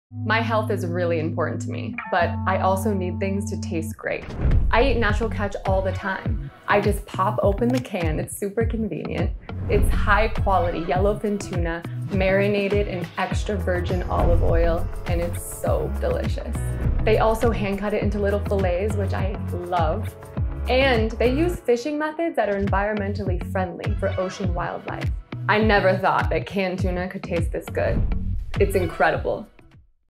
Highest Quality Canned Tuna On The Market
Does your Canned Tuna look like this? It should!
20G Protein 0G Carb 130 Calories

Hand Cut Yellowfin Tuna Filets marinated in Extra Virgin Olive Oil in an easy pop can. Unbeatable in taste, texture and presentation. A very high quality healthy source of protein. Keto & Paleo Friendly.

Every can of Natural Catch is sourced by Pole & Line Fishing which is a method used to catch tuna one at a time. NO NETS ARE USED OR ALLOWED. Pole & Line caught Tuna is much more expensive than any other method since it is all caught one at a time but is the best and safest method to protecting our oceans and wildlife such as Dolphins.

We do not care if it's more expensive to use these methods and we do not increase the price on our customers, instead we eat the cost to compete with the titan sized corporations who continue to cut corners on quality and use nets that hurt our oceans and wildlife because it's cost effective.

The goal of Natural Catch is to force the fishing industry and supermarket brands to stop using nets and switch to Pole & Line fishing by raising awareness of it's importance and at the same time producing an unbelievably high quality can of tuna to the market.

We are extremely proud of our product and are confident that our quality of Tuna is above all others. Open a can and see for yourself!